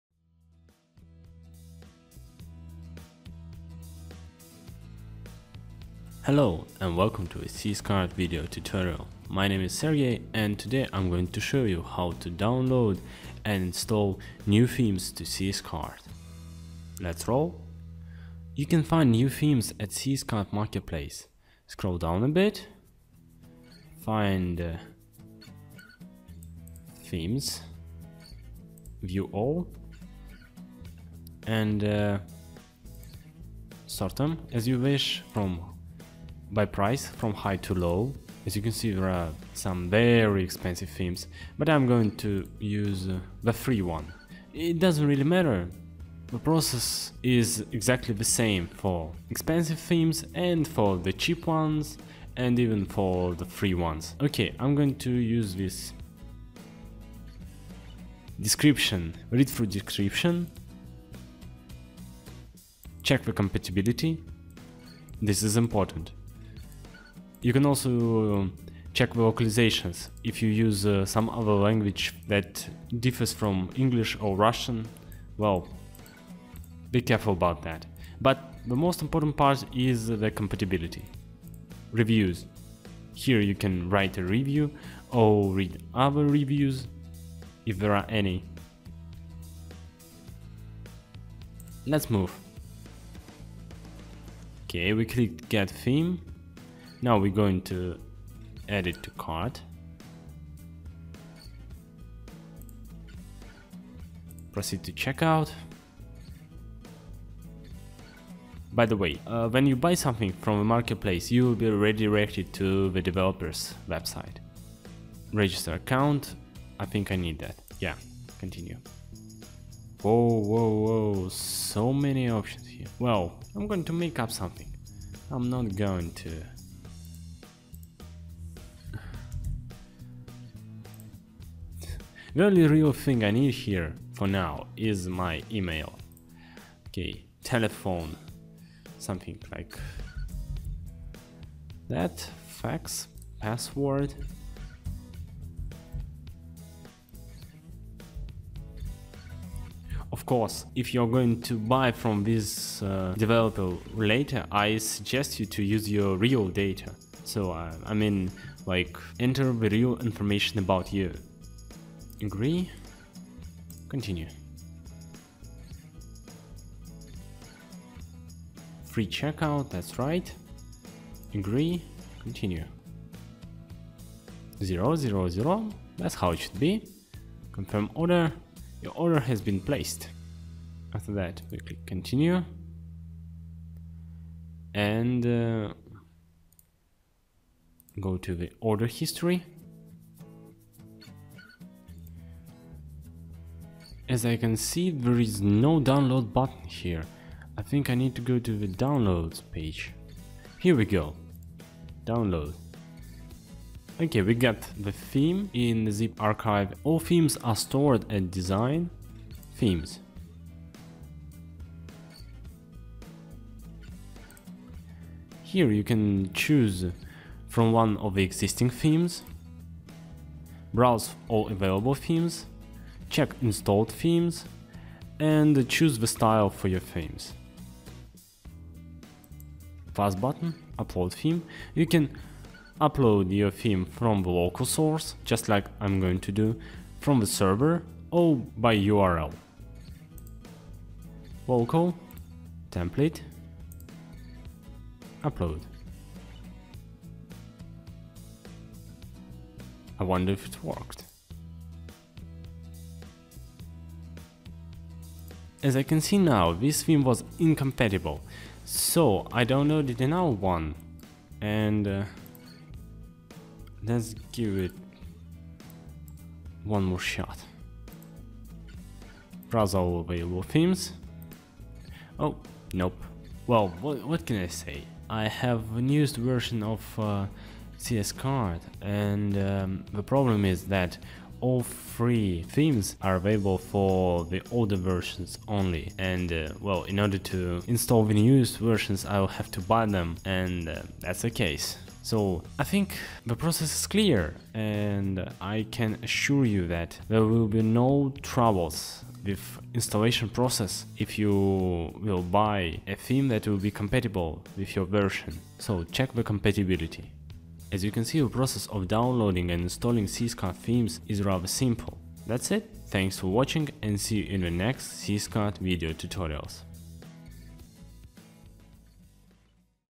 CS-Cart Tutorials. Getting Started with CS-Cart - How to Install New Themes in Your Online Store
This is a step by step instruction of CS-Cart theme installation process It takes only around 5 minutes to install a theme from CS-Cart Marketplace and the process is easy and intuitive.